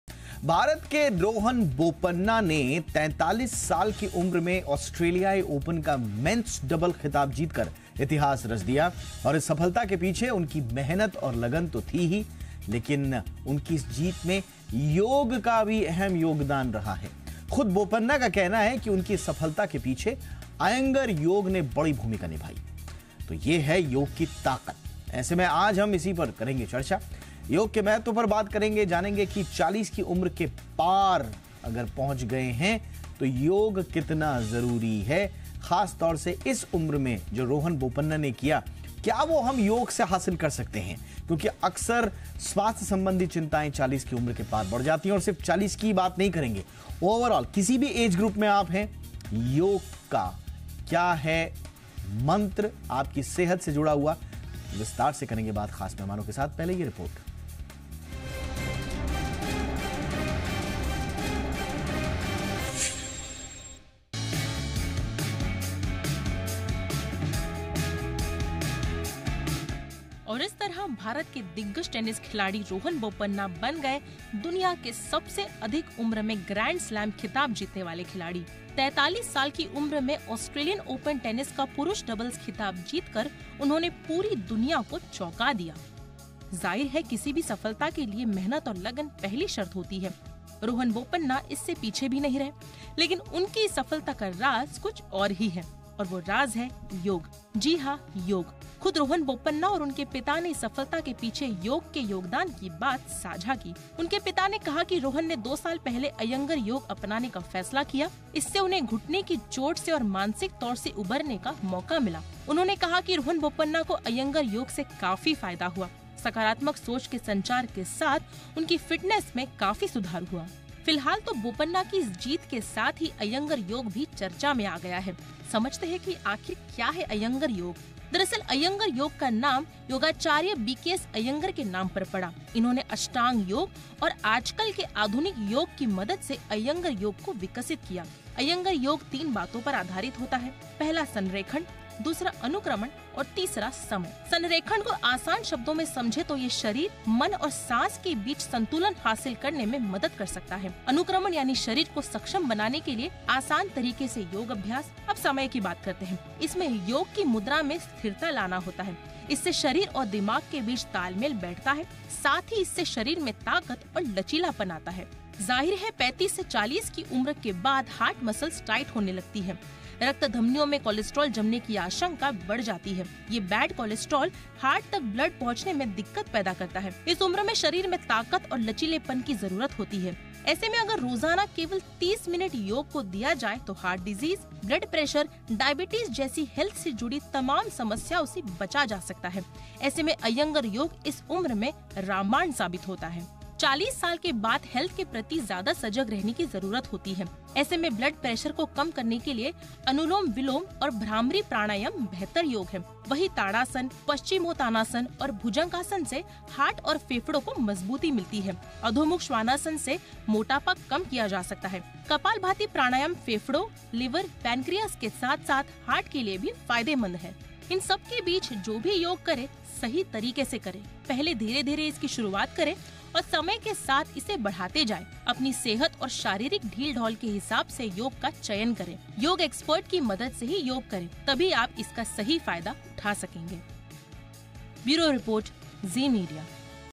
Yoga Beyond 40 : Yoga's Impact on Well-being After 40
आखिर 40 की उम्र के बाद क्यों और कितना जरूरी है योग ?
देखिए AapkiKhabarAapkaFayda की ये खास रिपोर्ट.